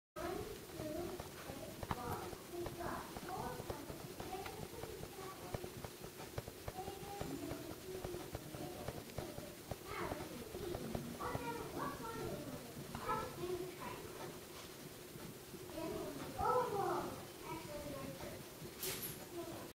My Cockatiel, Nippy
Here is a video of one of my other pets besides Milo the Shiba Inu. This is Nippy and he is a Whitefaced Cockatiel. He was my first pet. :)